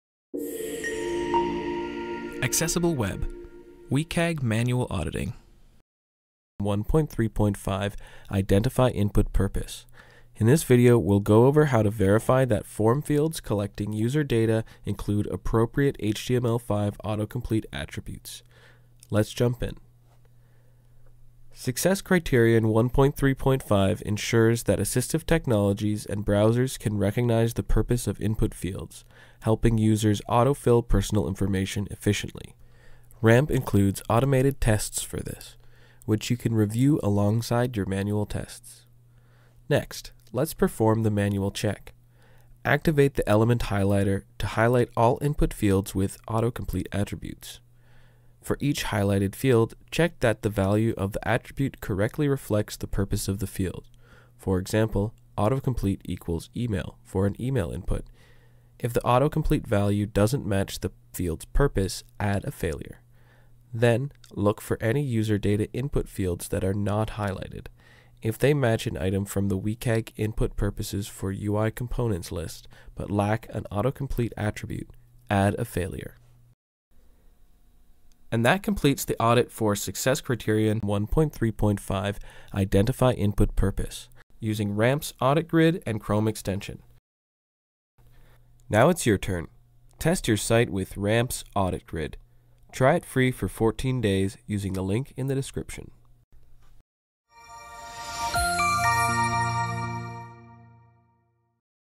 Testing 1.3.5 Identify Input Purpose | Manual WCAG Auditing Tutorial
In this video, we take a look at WCAG Success Criterion 1.3.5: Identify Input Purpose and walk through how to test it manually using the Accessible Web RAMP Audit Grid and our Chrome extension.

Our accessibility specialist shows you how to spot which fields this applies to, what to check for in the HTML, and how our tools make the process more straightforward.